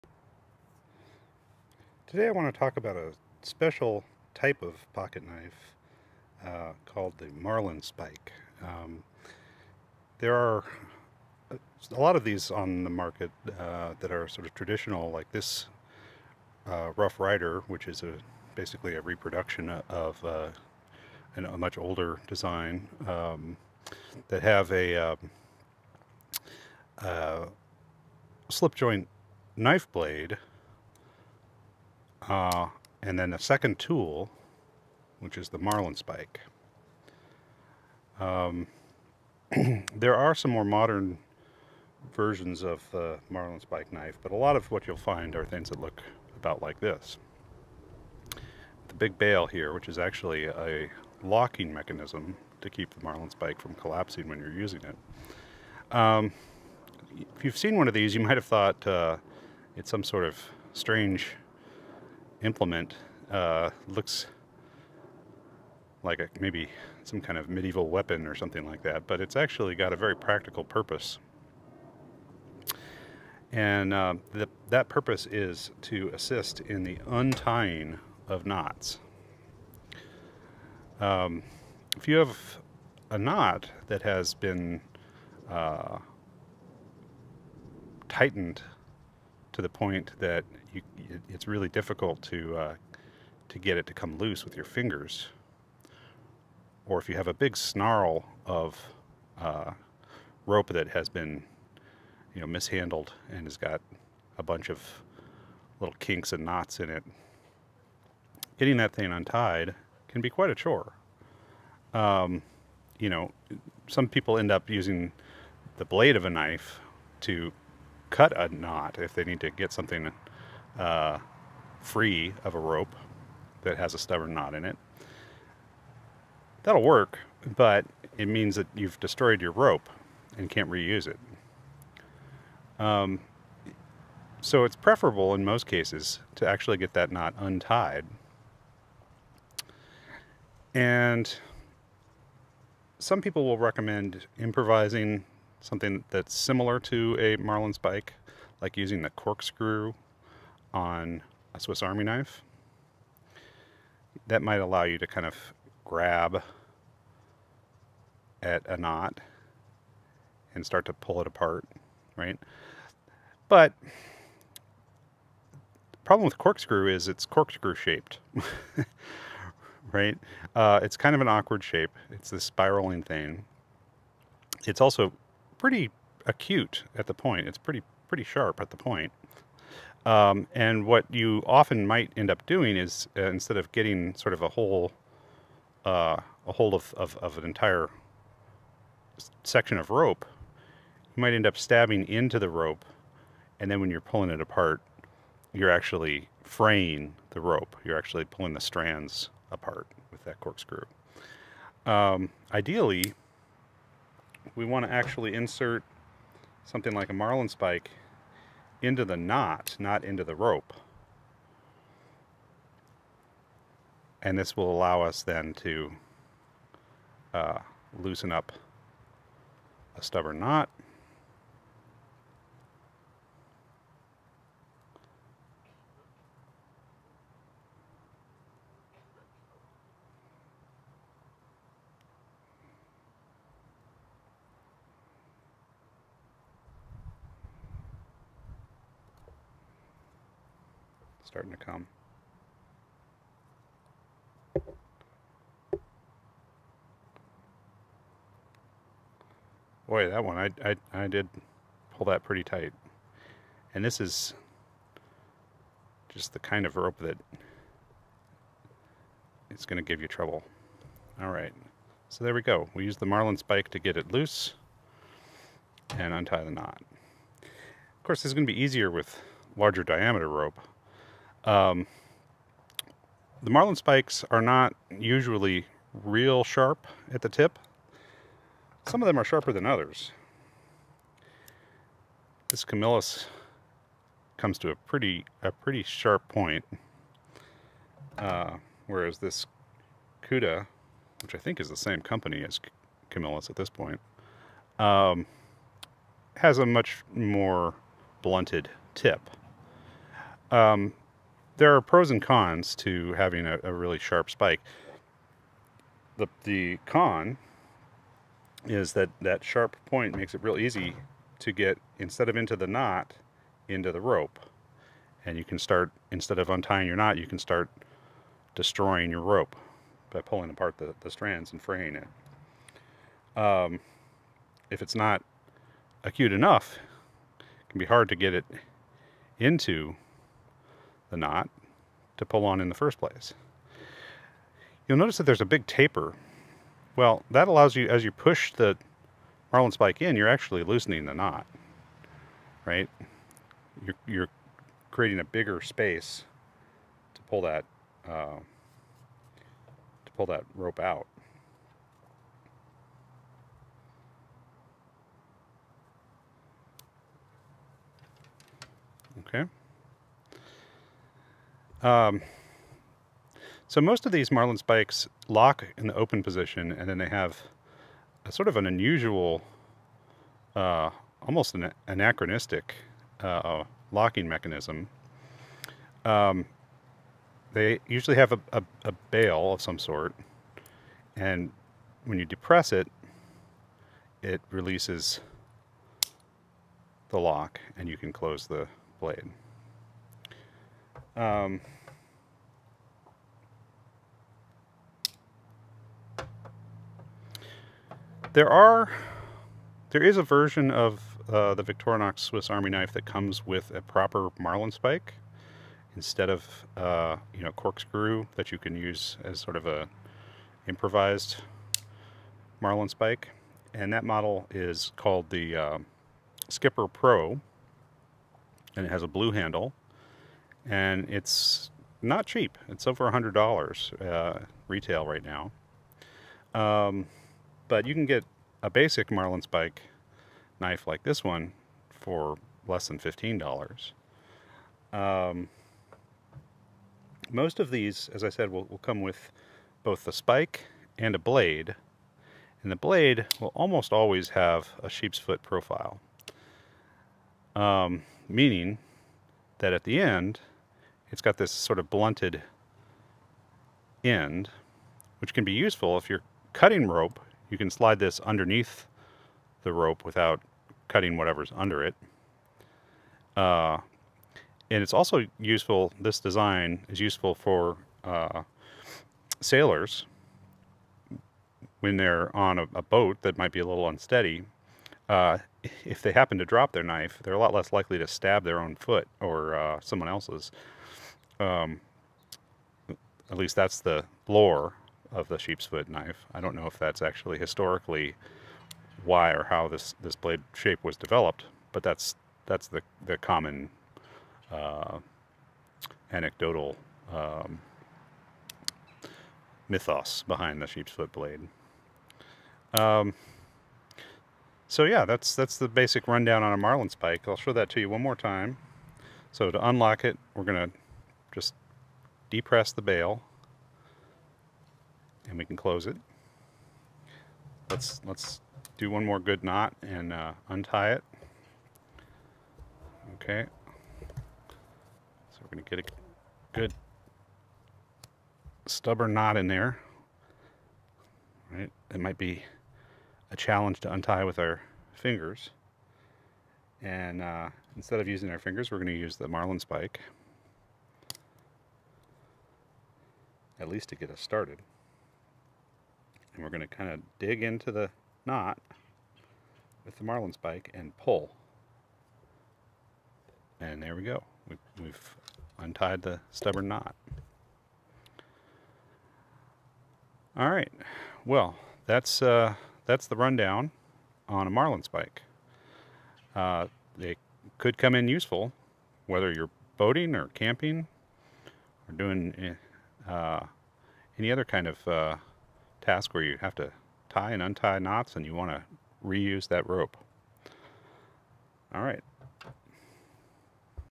The Marlin Spike -- a stubborn knot's worst nightmare!
It might look like some kind of torture apparatus, but the marlin spike is a very practical tool for any boater or outdoorsman.